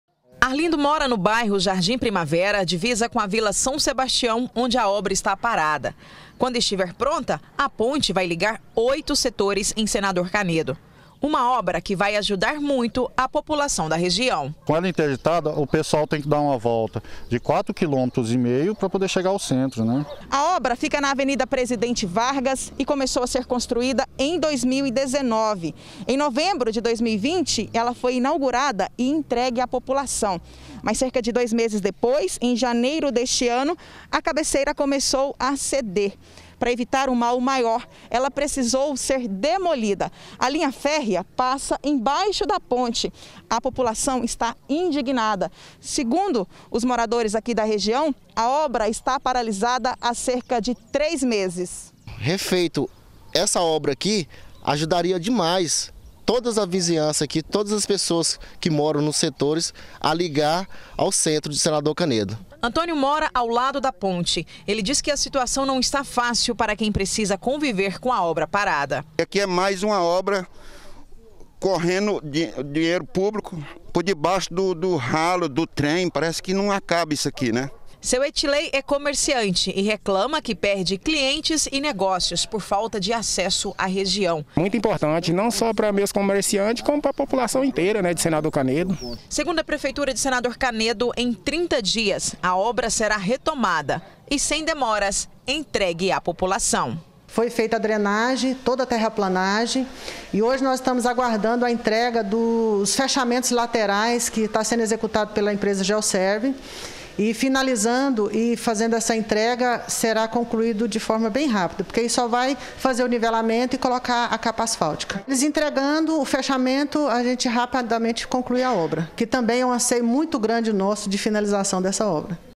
OBRA INACABADA: MORADORES COBRAM ENTREGA DE PONTE
OBRA INACABADA DE UMA PONTE EM SENADOR CANEDO TEM CAUSADO TRANSTORNO PARA OS MORADORES DA REGIÃO. ALÉM DE DIFICULTAR O ACESSO A VÁRIOS BAIRRO, O LOCAL TAMBÉM APRESENTA RISCO AOS MORADORES.